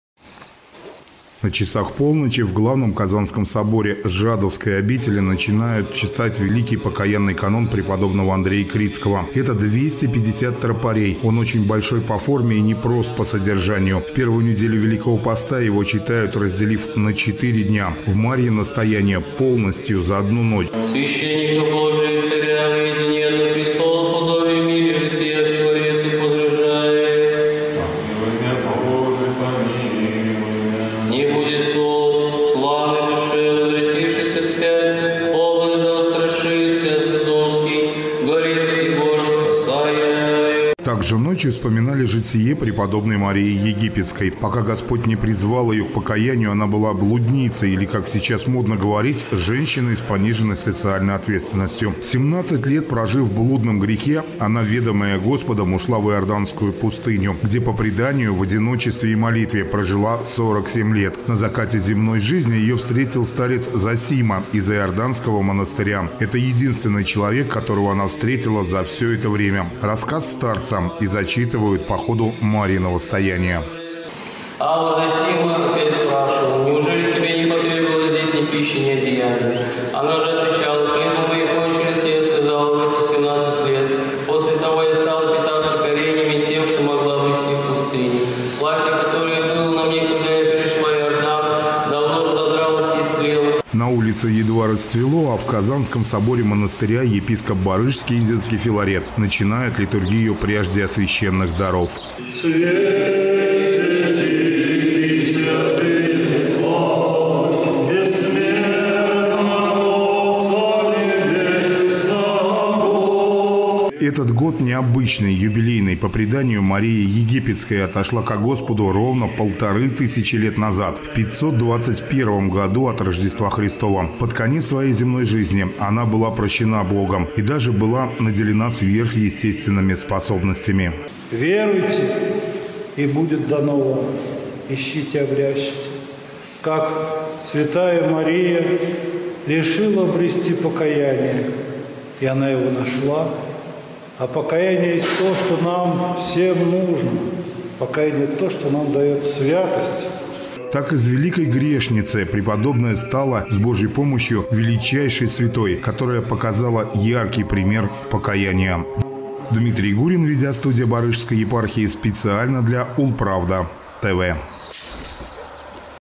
Православные ульяновцы провели молитвенный марафон
Около восьми часов, с небольшими перерывами. Самый продолжительный великопостный молитвенный марафон называется «Мариино стояние». С полуночи четверга в церквях и храмах православные вспоминают молитвенный подвиг преподобной Марии Египетской, которая около полувека прожила в пустыне, замаливая свои блудные грехи. Венчает марафон литургия преждеосвященных даров, которую в Жадовском монастыре возглавил епископ Барышский и Инзенский Филарет.